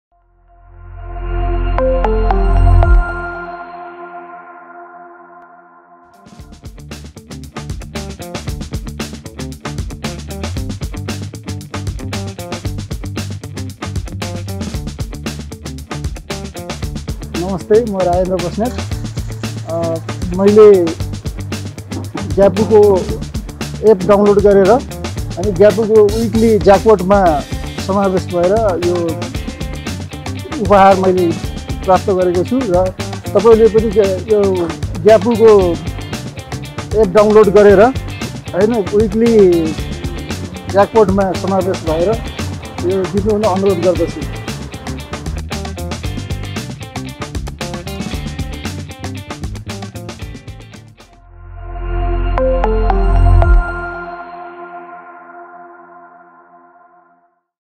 Prize redemption of Weekly Jackpot. Special Prize Portable Juicer and Blender.
Exclusively on gyapu mobile app.

Log into gyapu mobile application for Weekly Jackpot
Book exciting products at Rs. 0 and get a chance to take it home absolutely free.